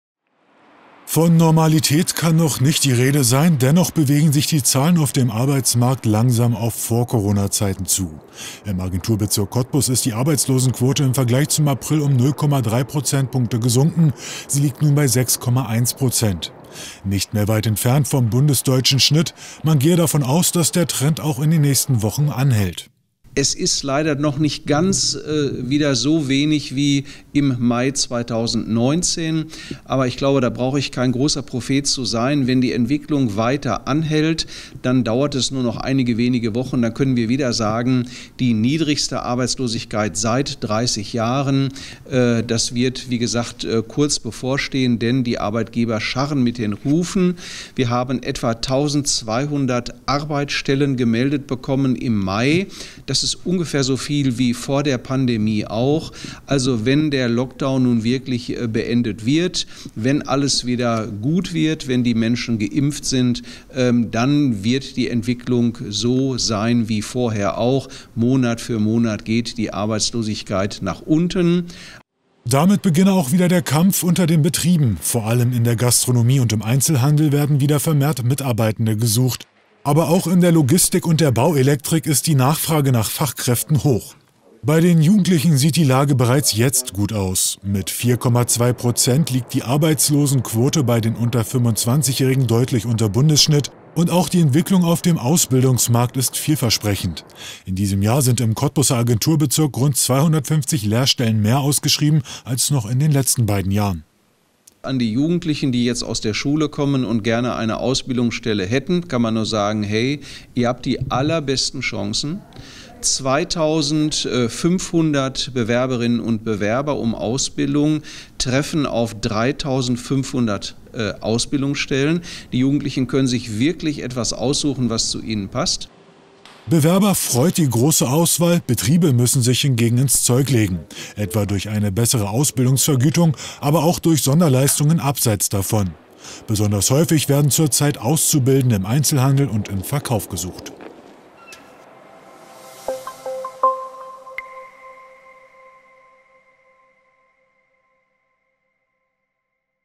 Ein Schritt in die richtige Richtung - der Arbeitsmarkt im Mai 2021
Im Agenturbezirk Cottbus ist man zwar noch nicht auf dem Niveau, dass man vor der Pandemie hatte - die Richtung stimme aber zumindest. Die Zahl der als arbeitslos gemeldeten Personen sinkt. Man erwartet, dass sich die Lage weiter entspannen werde, wenn es zu weiteren Lockerungen kommen sollte. Für Schüler auf der Suche nach einem Ausbildungsplatz sei die Situation hingen so gut wie lange nicht mehr.